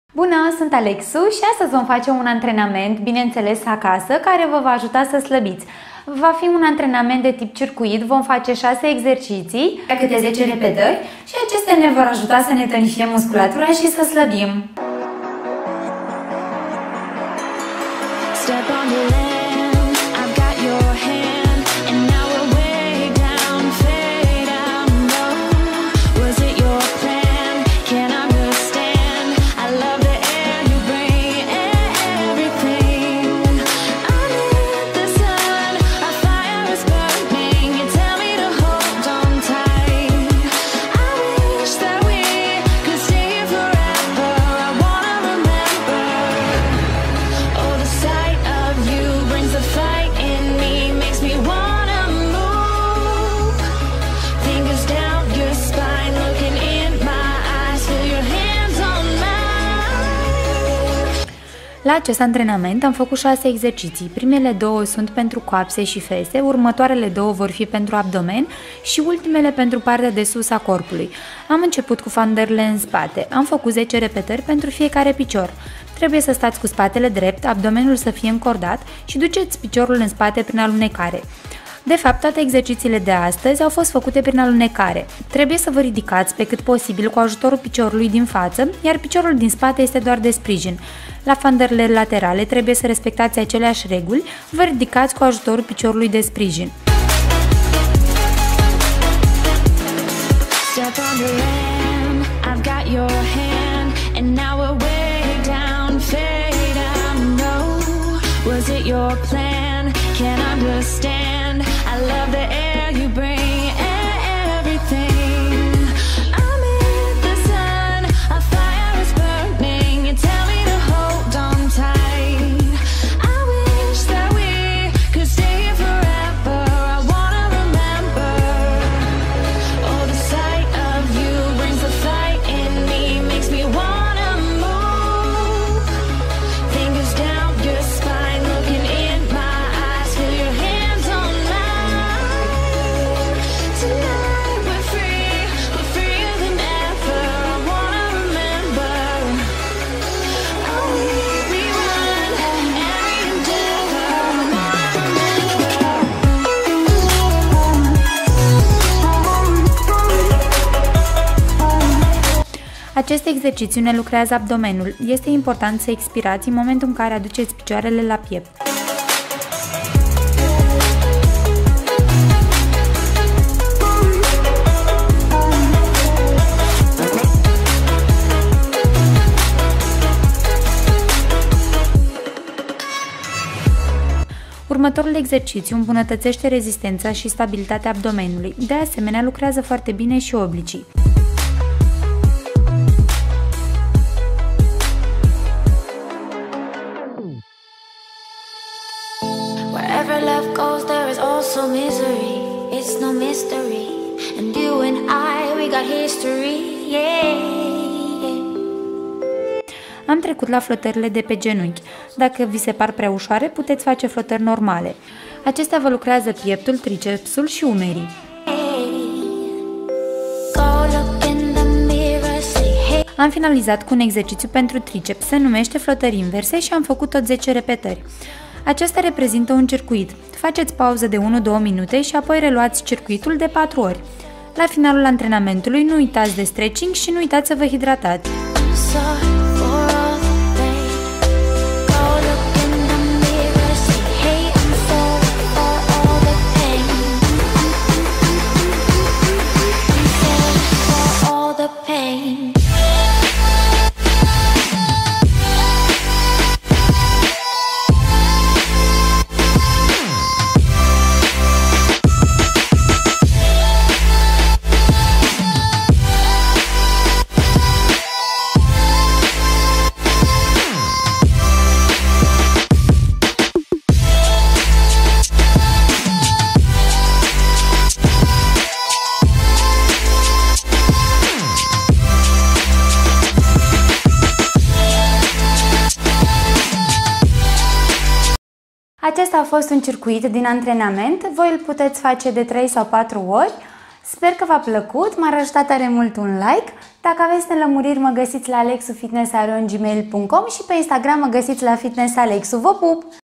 Slabeste Acasa in 10 minute ! #11
Am facut 6 exercitii a cate 10 repetari fiecare si acestea formeaza un circuit .
Nu uitati sa va hidratati si spor la antrenamente !
Aceste antrenamente va ajuta sa slabiti si sa va tonifiati .